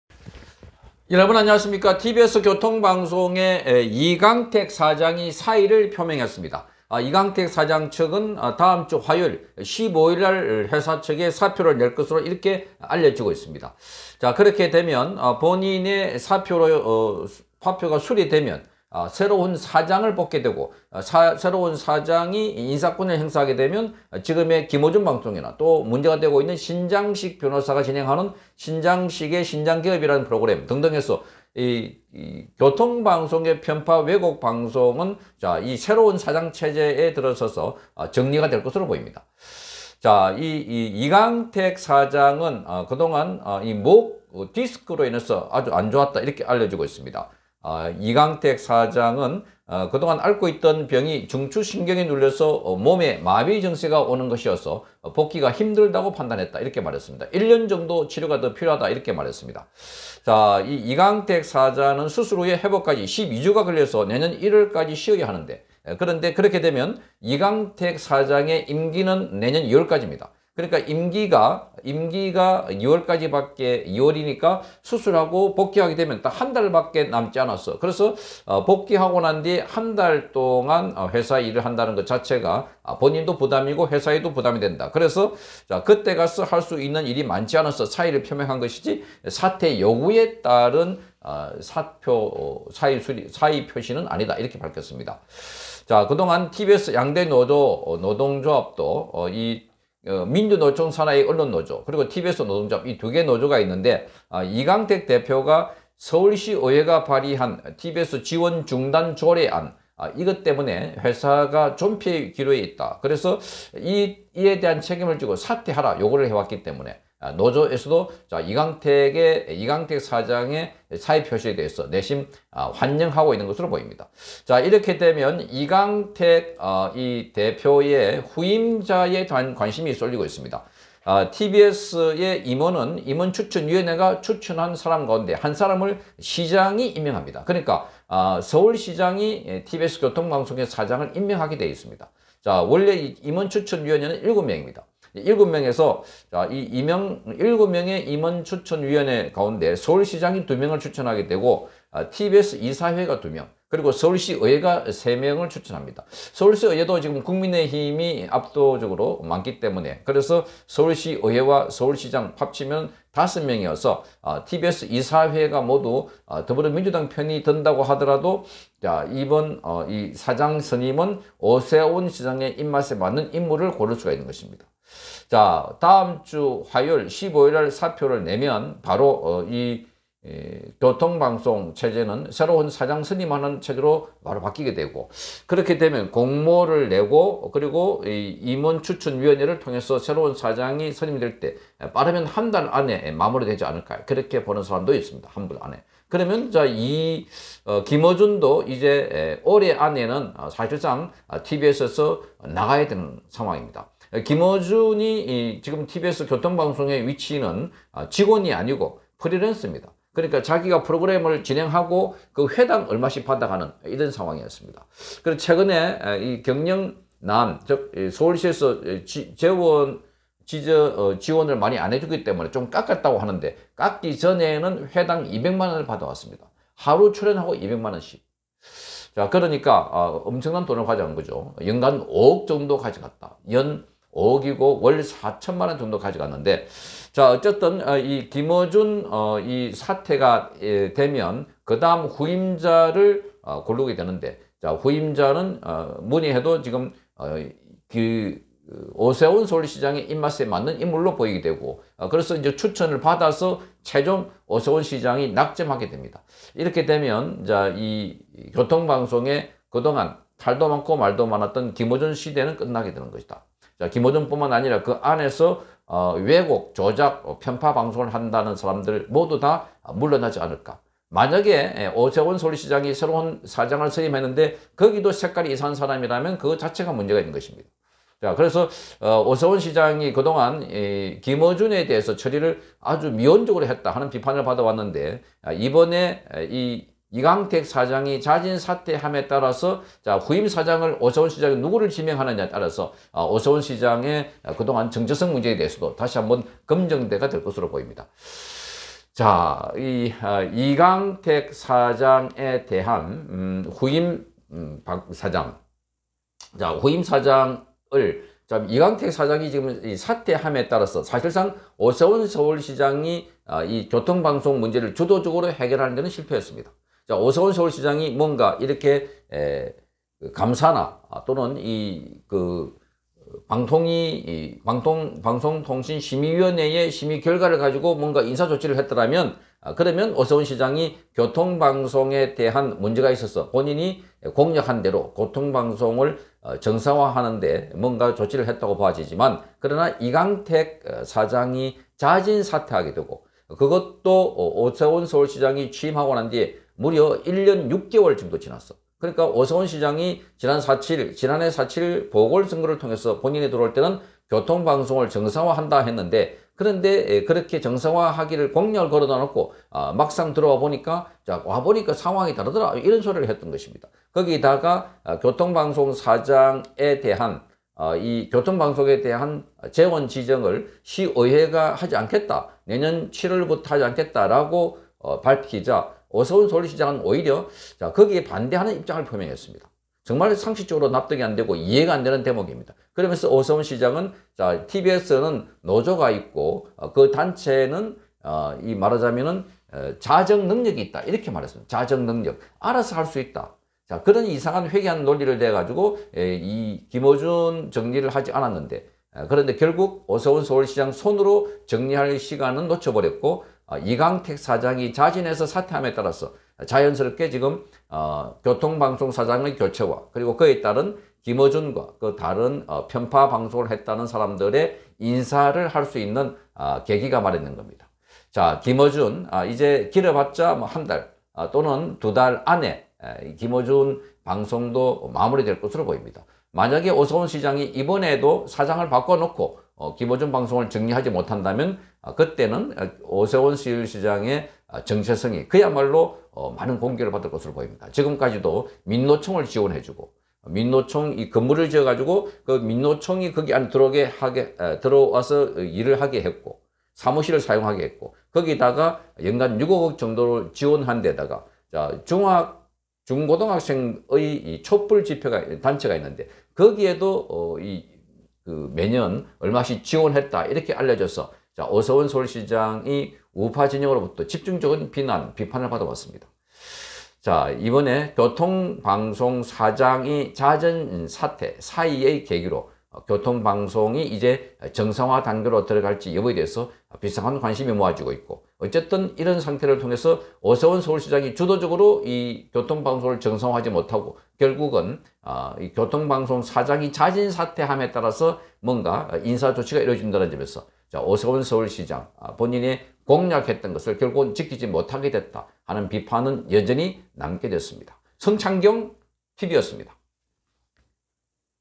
서울시 " 이강택 사장 사퇴하면, 임원추천위 열어 후임 사장 인선" 이강택 "15일 사표 제출 할 것"
7인의 임원추천위원회가 사장 추천하면 오세훈 시장이 임명. 7명의 위원은 시의회 3명, 오시장 2명,현 이사회 2명 추천. 오시장이 원하는 사람 사장에 선임할 수 있다.